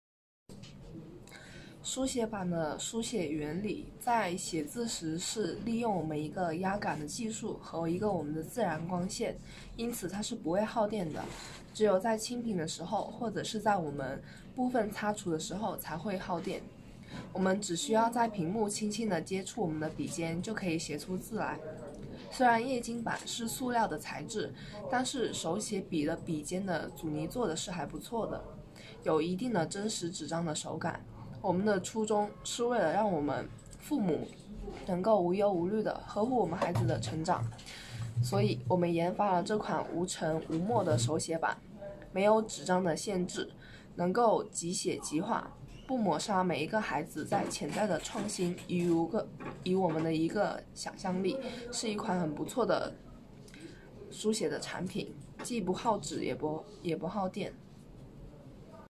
LCD handwriting board, understand the composition of this product
The external structure of the handwriting board, which is usually what we call the shell material, the external structure, can be used as a product through the development of appearance molds and injection molding. It is a liquid crystal handwriting board suitable for children's drawings and adult notes.The important part of this product is our liquid crystal film. From the picture, we can understand that the liquid crystal film handwriting board is divided into three major components, the upper and lower two layers are plastic protective films, which are the same material, and the middle The liquid crystal is injected into the sealed state, and then the liquid crystal can be exposed to the pressure of the hard object by hydraulic pressure, so that writing, drawing and writing can be performed. The outer surface part is a fixed structure, which is composed of a very viscous surface glue, through which the liquid crystal film and the shell material can be firmly combined. Only then can it be used by customers.The internal structure is the heart of this product, which is what we often call the control panel. Why this product can write more than 50,000 times is definitely inseparable from the user’s repeated one-click erasing, so here is the function of the control panel. Through the control panel, a voltage inside the liquid crystal The internal instantaneous discharge can achieve the purpose of dilution, thereby instantaneously separating the adhered liquid crystal parts, thus achieving the purpose of cleaning. It becomes a piece of white paper that has never been written on.